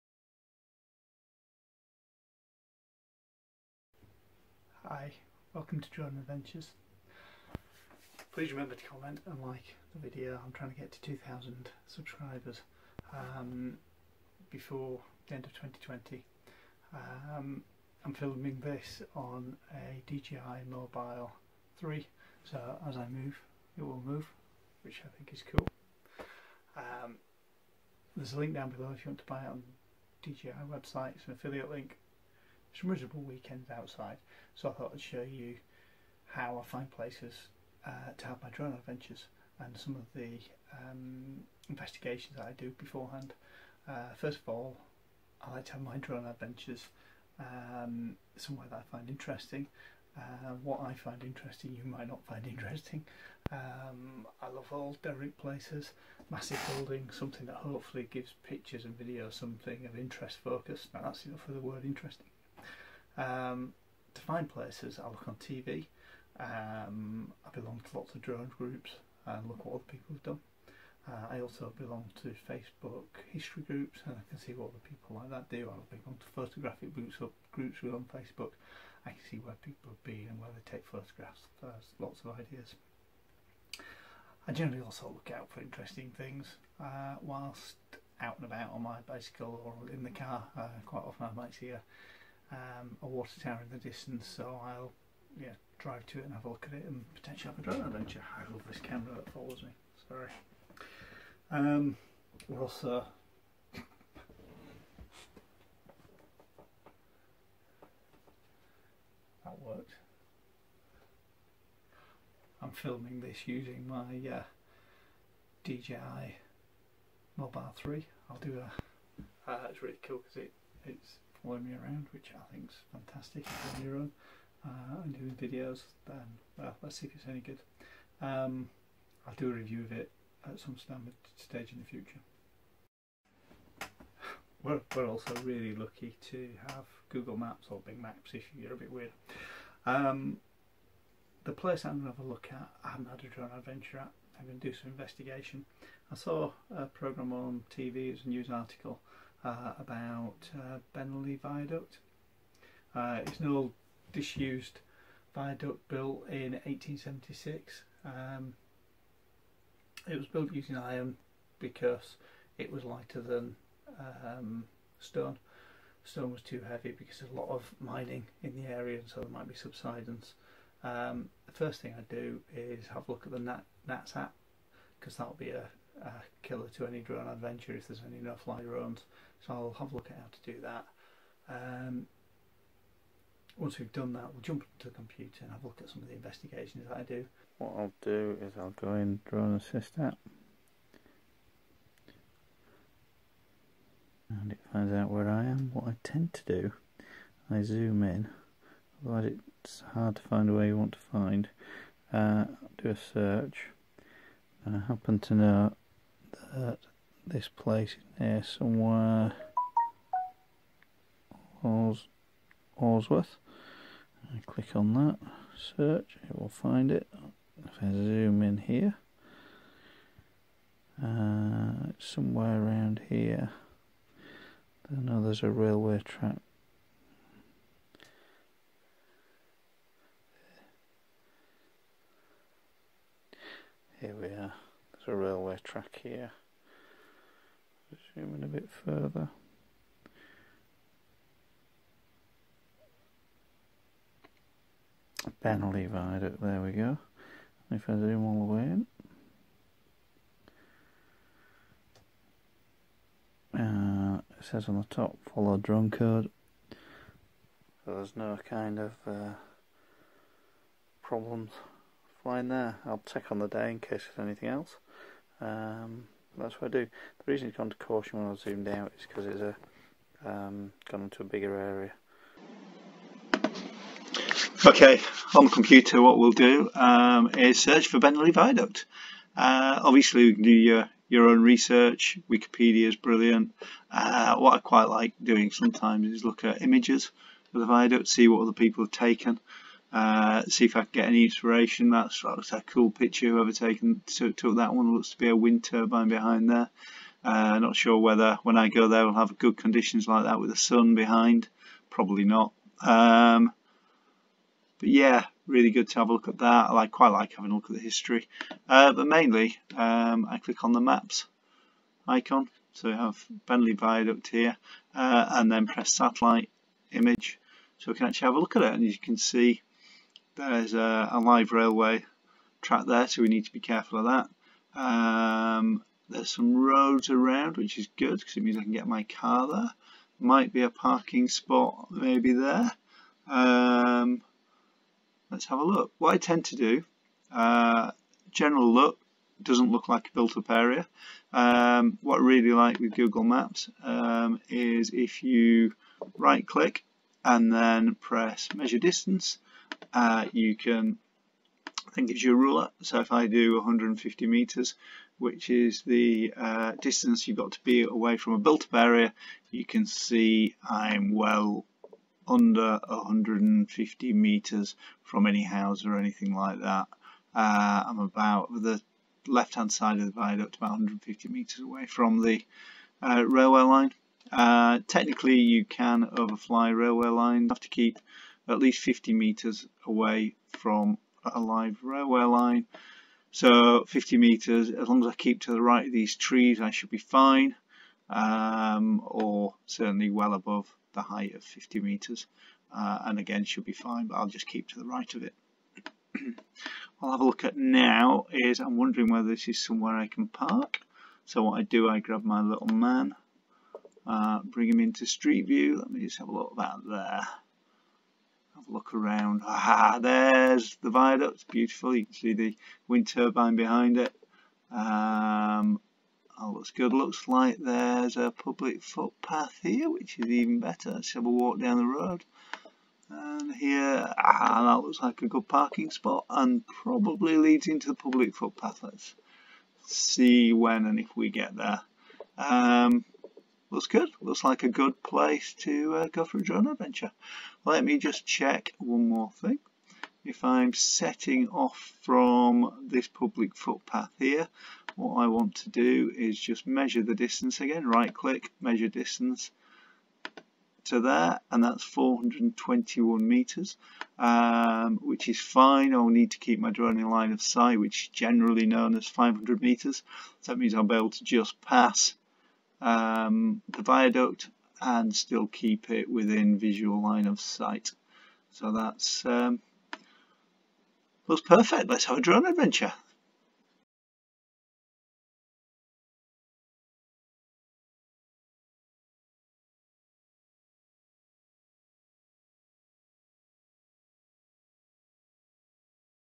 Planning a drone flight (Drone Adventure) - Bennerley Viaduct
In my video, I demonstrate my process for planning a drone flight which includes checking NATs applications and researching distances on Google Maps.

Although the practice of checking distances and whether it is legal to fly a drone is still good practice, please note that Bennerley Viaduct is now accessible by foot, so will not be much easier to access and fly.

I hope you found this useful :-)